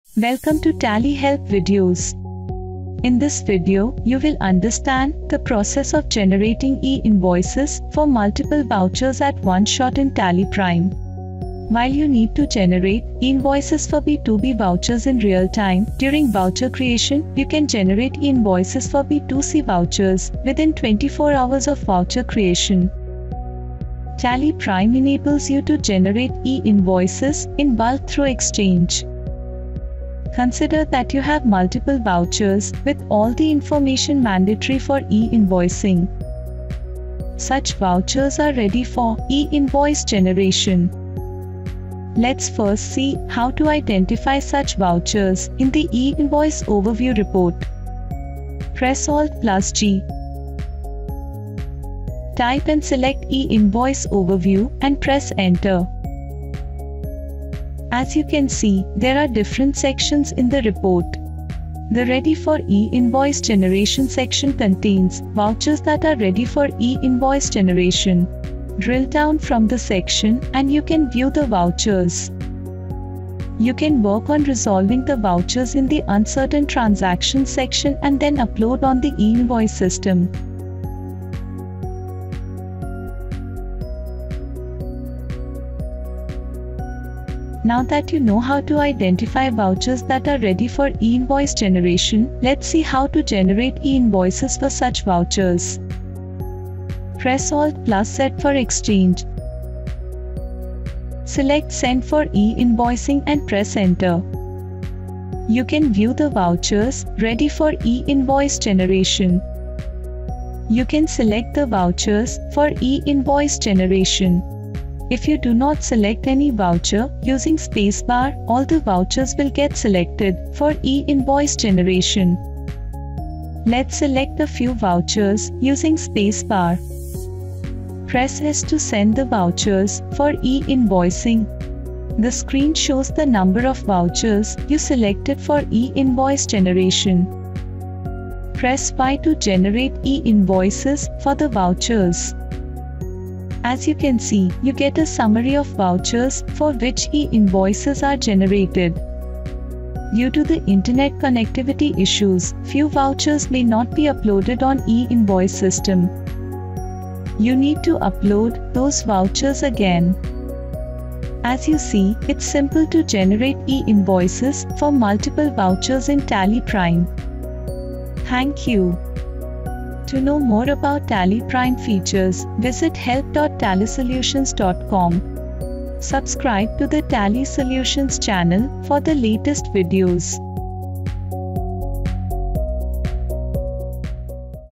How to Generate Bulk e-Invoice in TallyPrime | TallyHelp
For B2B vouchers, e-Invoices must be generated instantly as you create the vouchers. However, for B2C vouchers, you have a 24-hour window to generate e-Invoices. Given the volume of B2C vouchers processed daily, consider generating e-invoices for all vouchers in a single batch to streamline the process.

This video demonstrates the process of generating e-Invoices for multiple vouchers.

The video covers:
00:00 Title and Introduction
00:42 Identify vouchers ready for e-invoice generation
01:28 Generating e-invoices for multiple vouchers